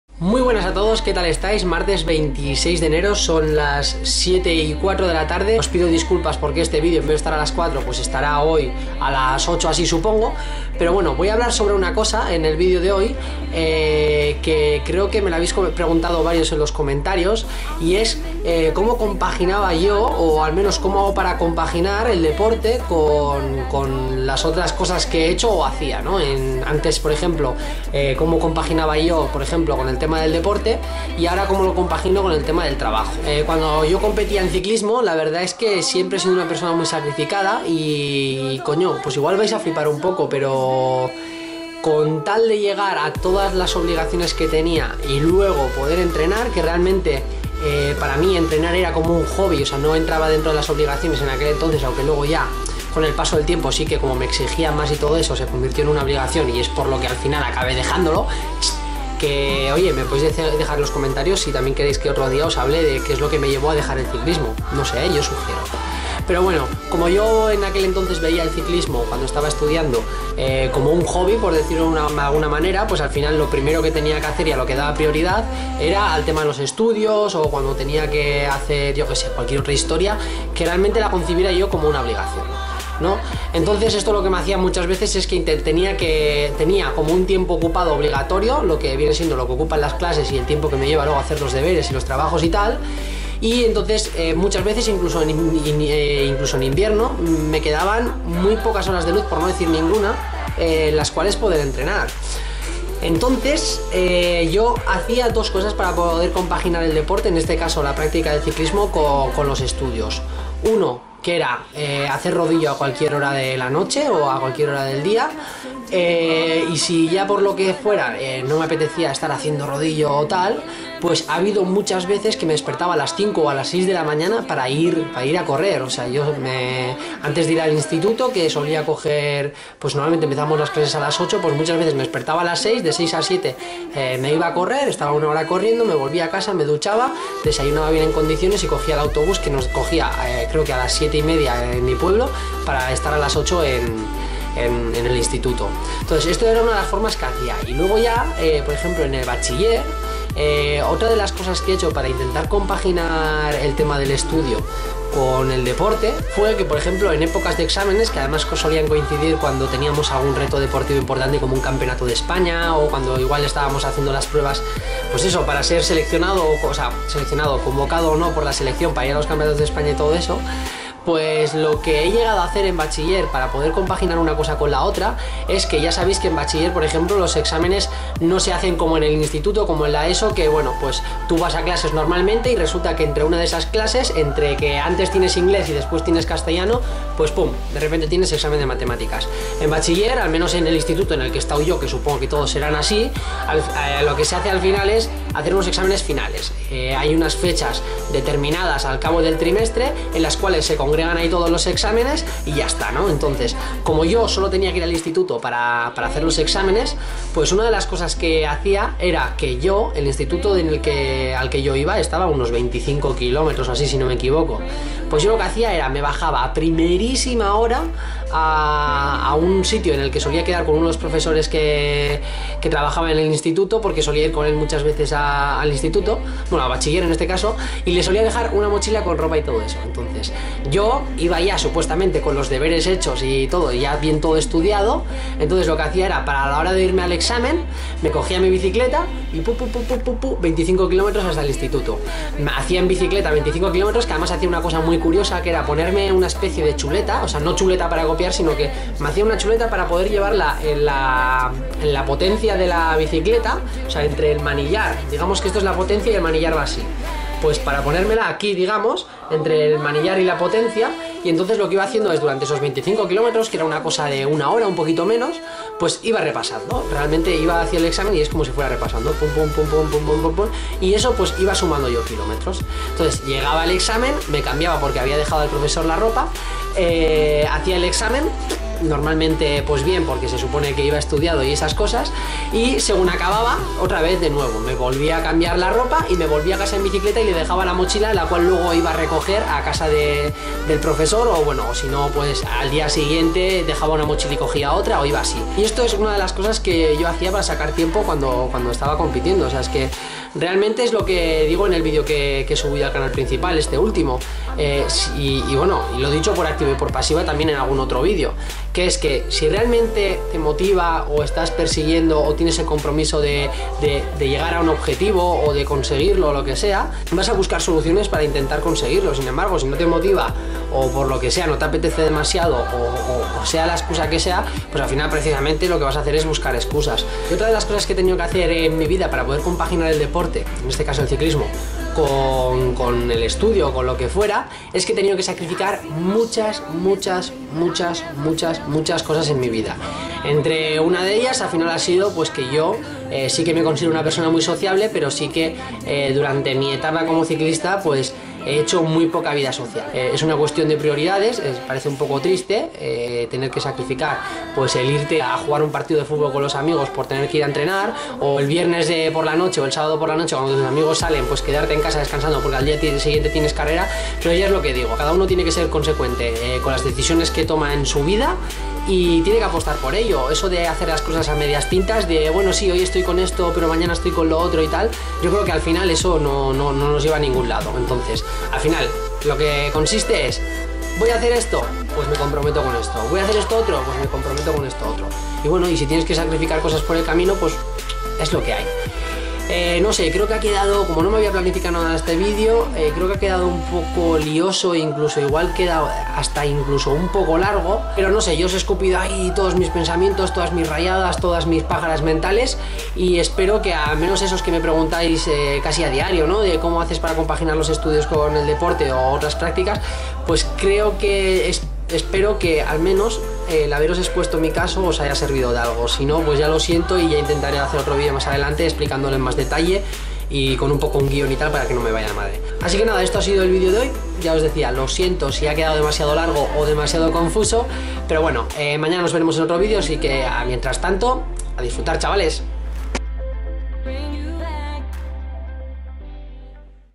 Cómo compaginar el deporte con los estudios y el trabajo
En el vídeo de hoy os explico cómo hacía yo para compaginar el deporte con el resto de actividades como los estudios o el trabajo. Al final se ha hecho un vídeo un poco largo y lioso, pero espero que aún así os sirva.

A ver si para mañana podemos volver al ritmo normal de publicación, que estos días voy hasta arriba de trabajo y voy a lo que doy.